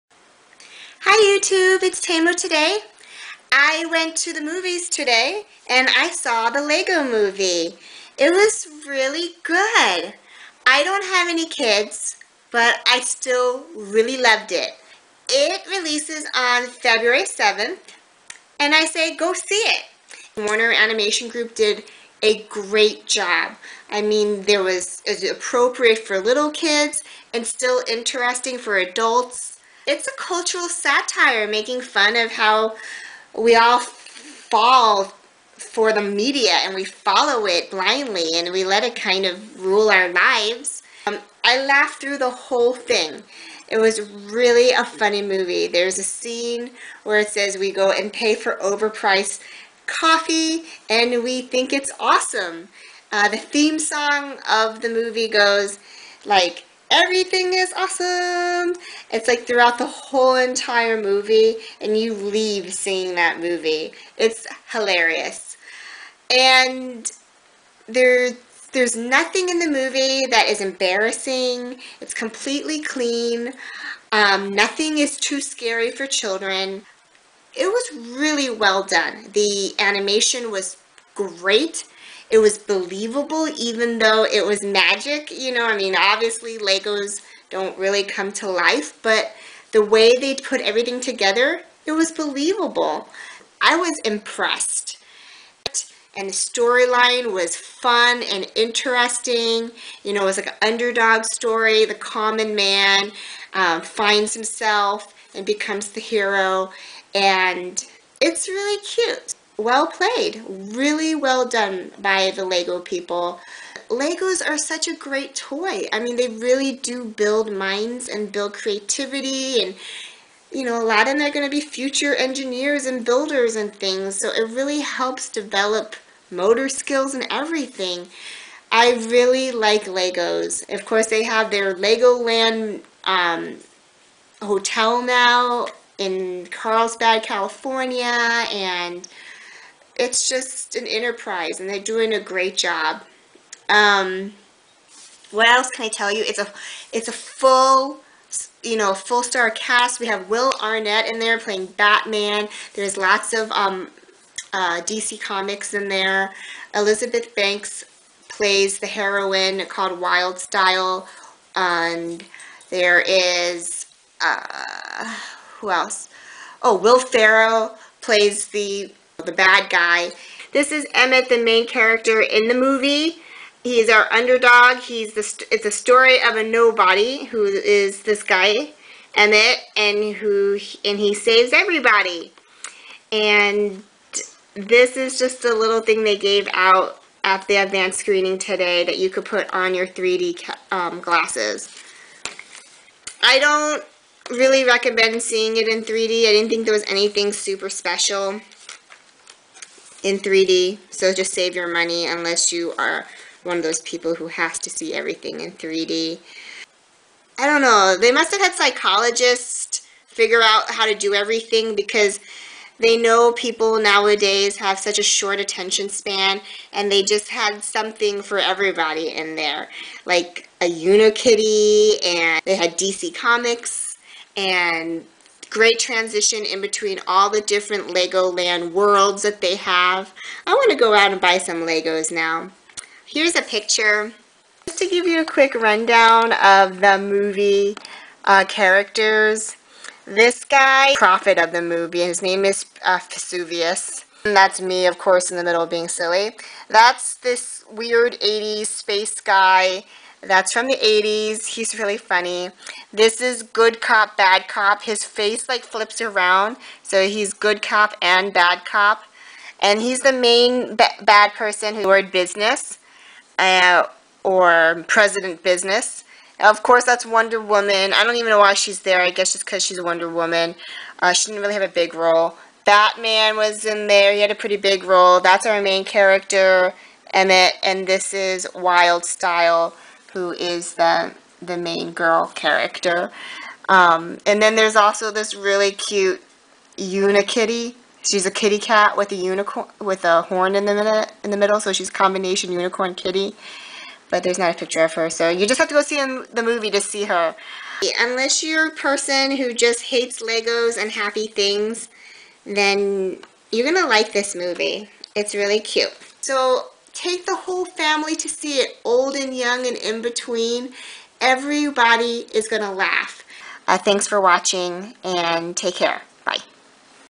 Movie Review: The Lego Movie-- Assembling February 7
Went to an advance screening of The LEGO MOVIE.  Here's Tamo Today's take!